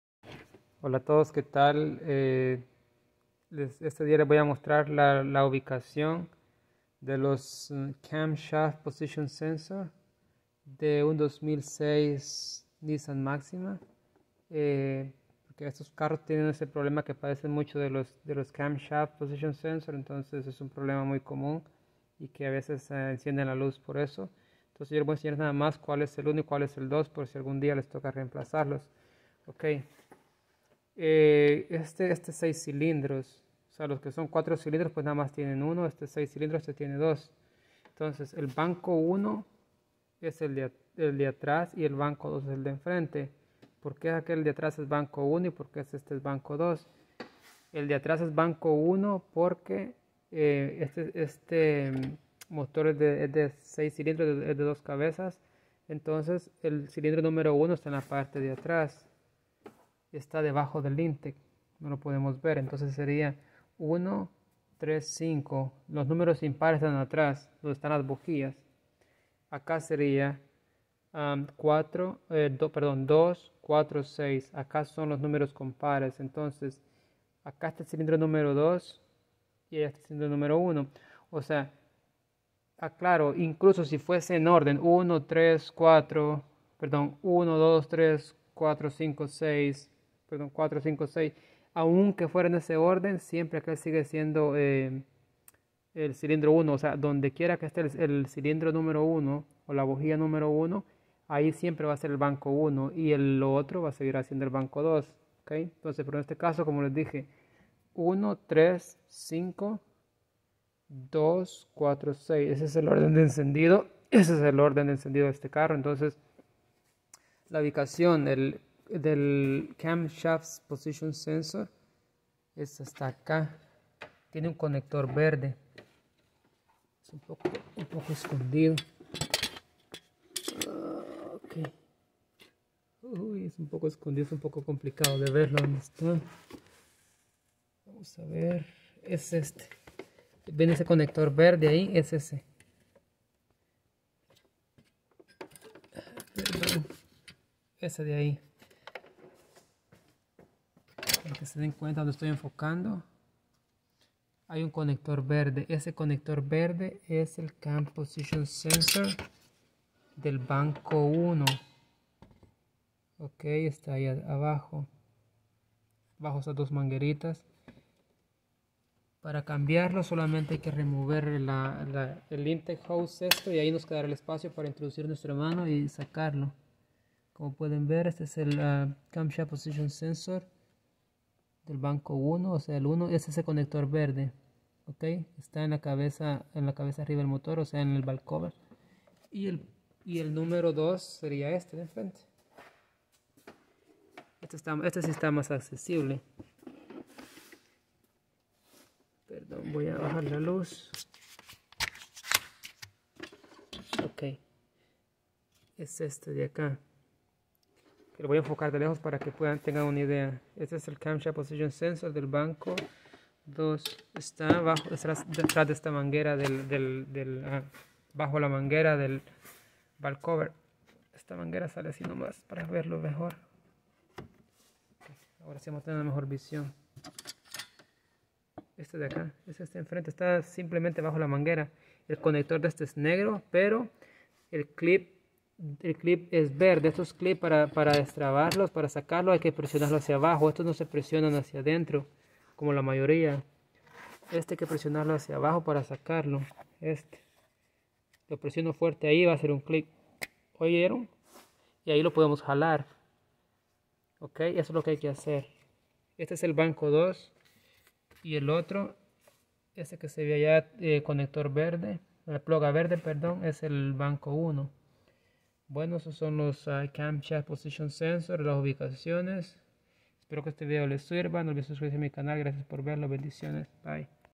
2006 NISSAN MAXIMA 6 CILINDROS, UBICACIÓN DE LOS CAMSHAFT POSITION SENSORS
Mecánica Automotriz